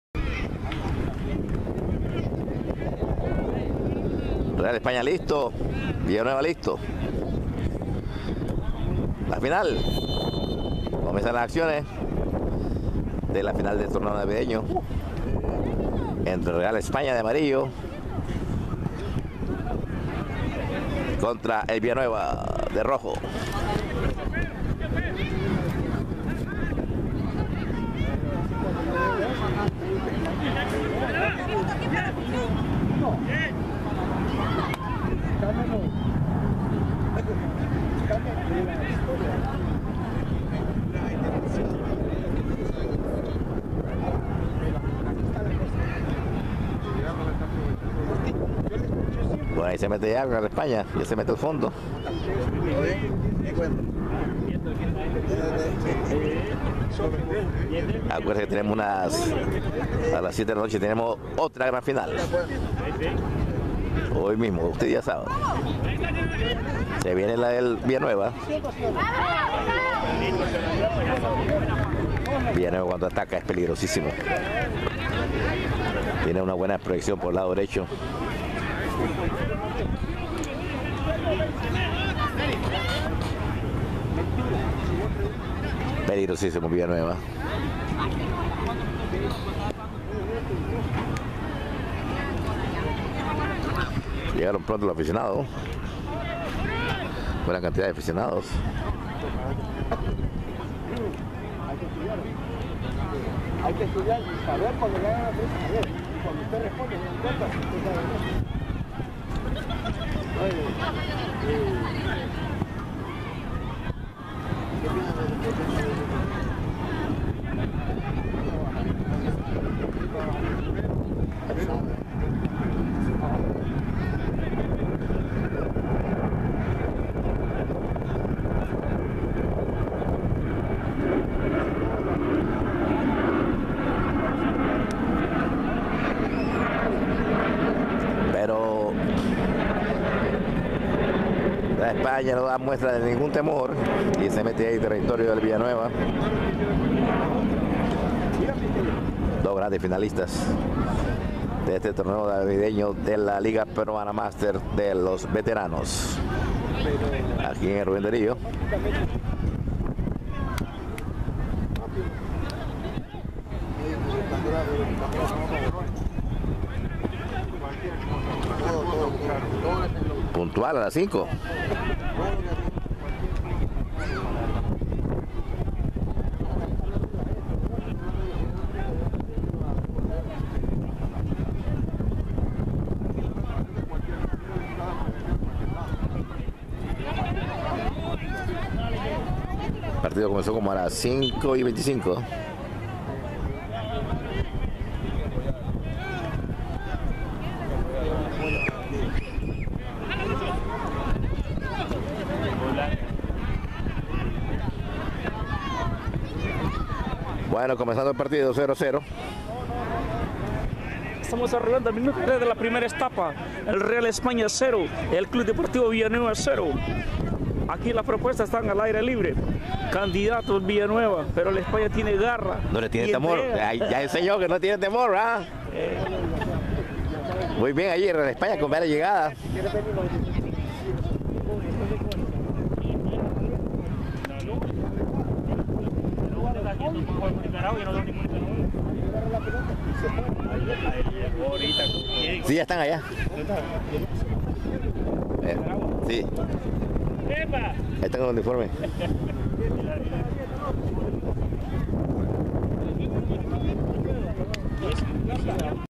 Villanueva vs Real Espana La Final
Villanueva vs Real Espana La Final Torneo Navideno Liga Peruana Master parque Ruben dario Miami Florida 24 de Enero del 2015.